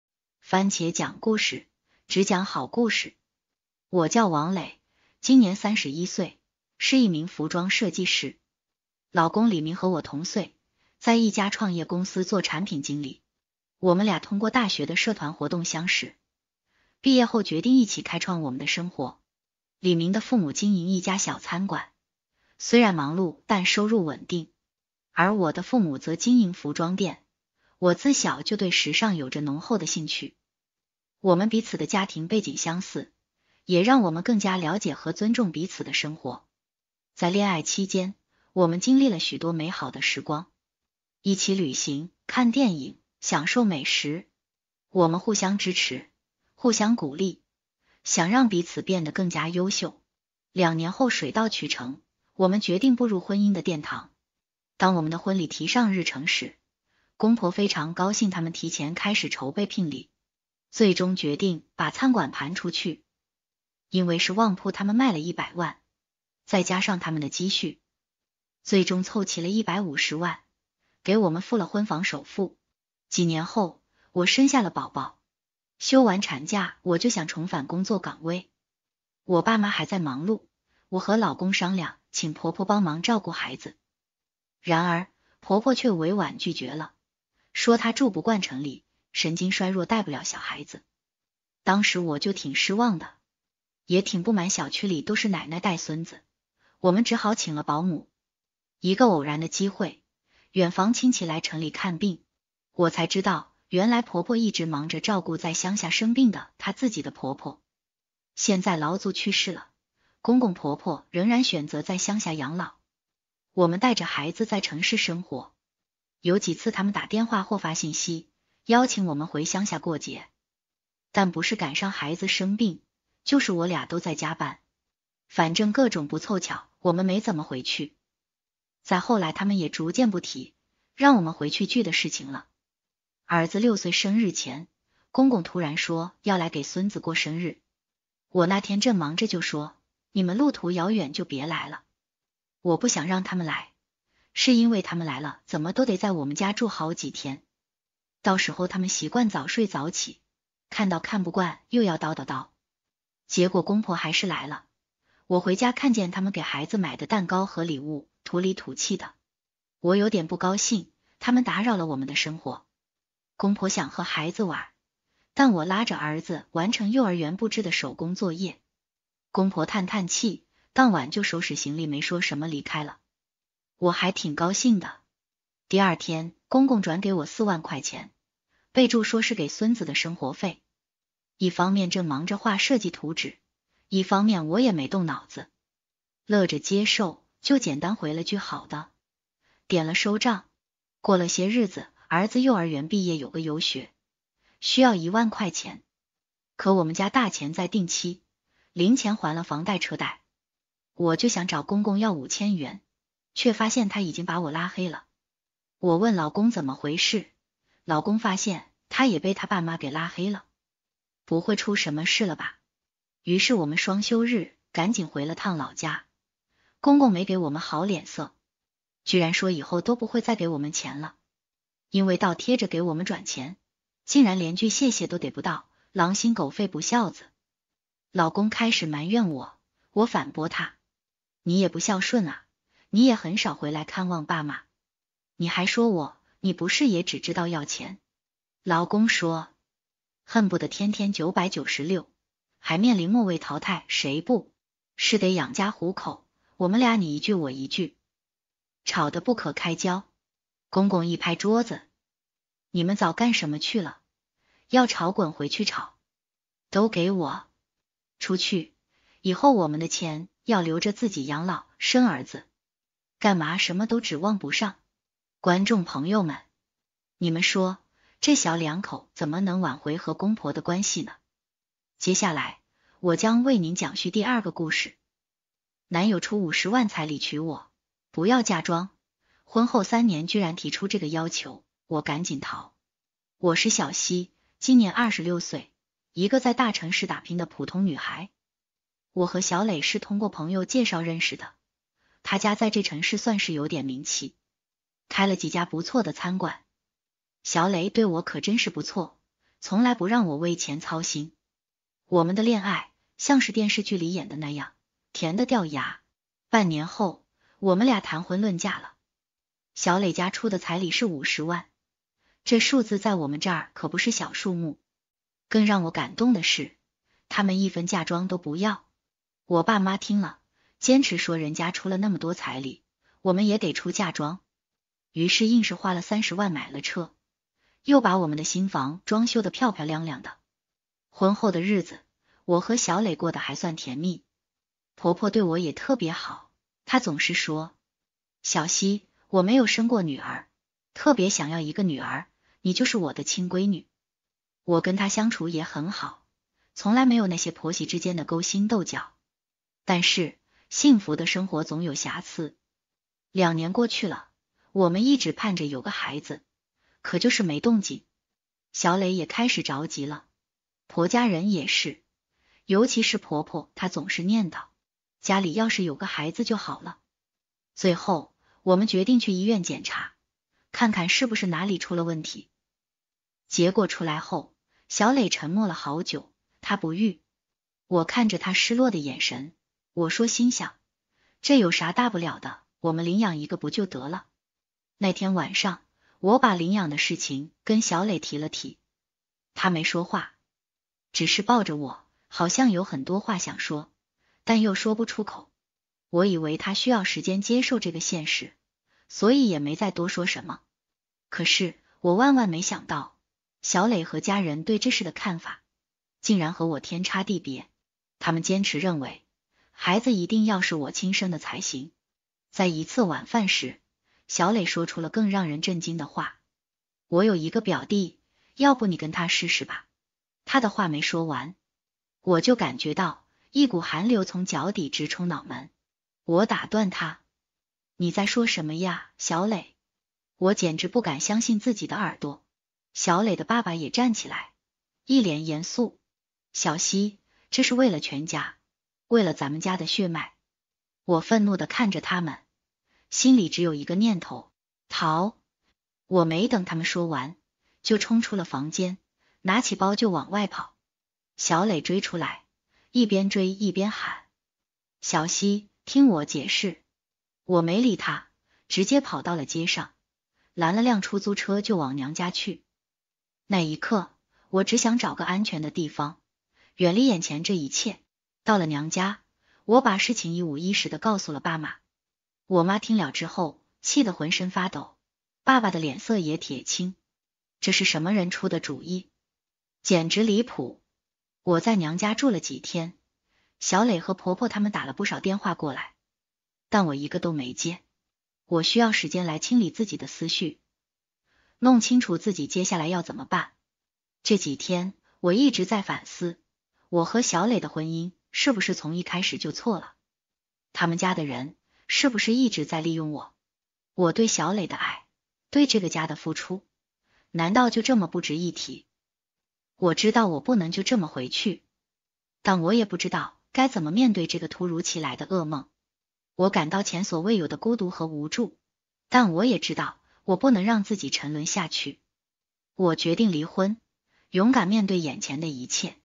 公婆卖了餐馆给我们付首付，却因为一件小事，竟把我和老公都「拉黑」了！
本期視頻制作人員：
文案/策劃：小松
剪輯/字幕：小天
校對/後期：夏明